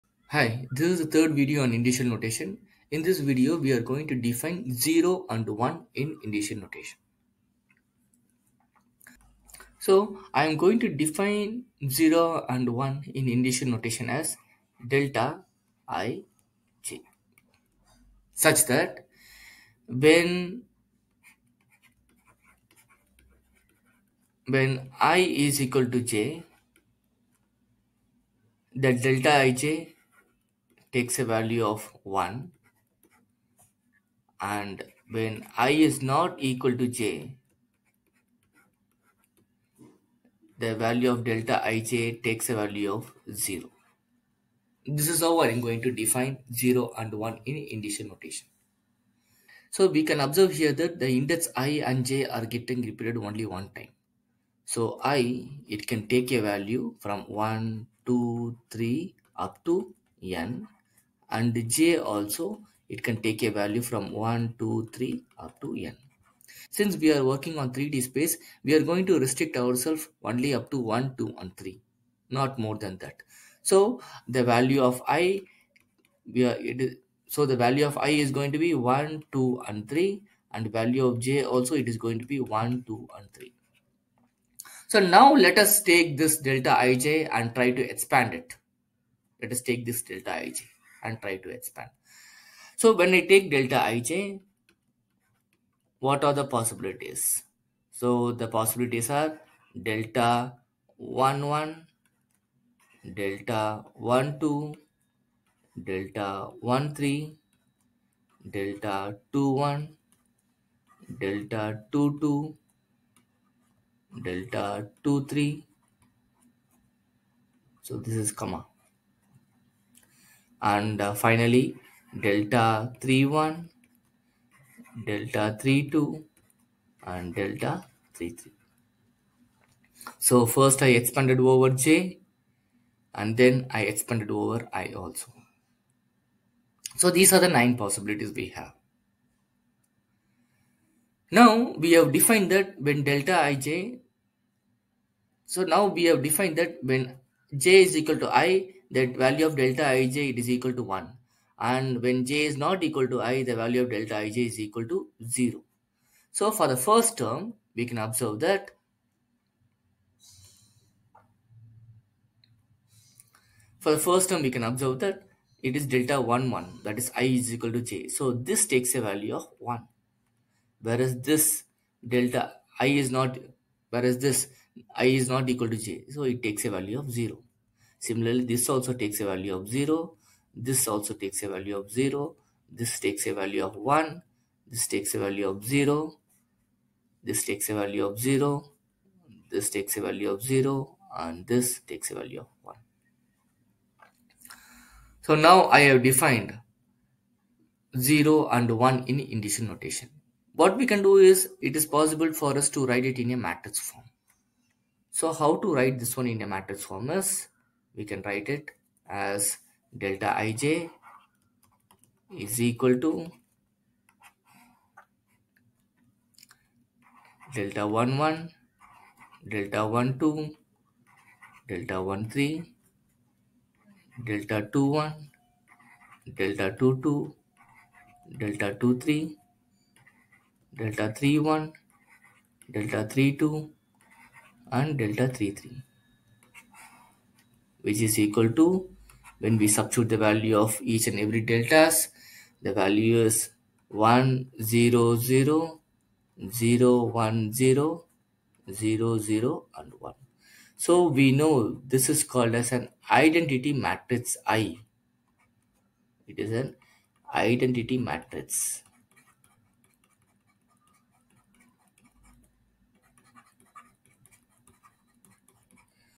Lecture 3 - Introduction to Indicial Notation
Hi,
In this video, you will learn how to represent 0 and 1 in indicial notation.
The 0 and 1 are represented in as an Identity matrix [I] in tensorial form.